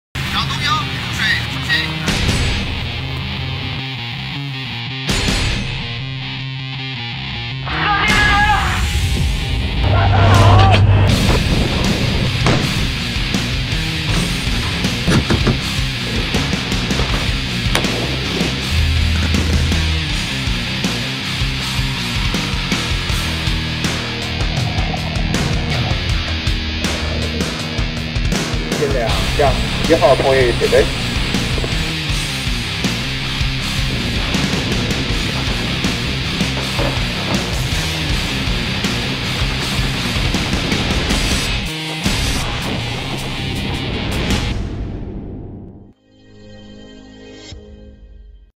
Hải quân Trung Quốc huấn luyện diễn tập đổ bộ đường biển quy mô lớn có sử dụng đạn thật
Video mô tả một cuộc diễn tập đổ bộ quy mô lớn của Hải quân Trung Quốc với sự tham gia của nhiều thành phần, các loại vũ khí như lực lượng người nhái, xe tăng lội nước, máy bay trực thăng có vũ trang,.

Source image in video: PLA Navy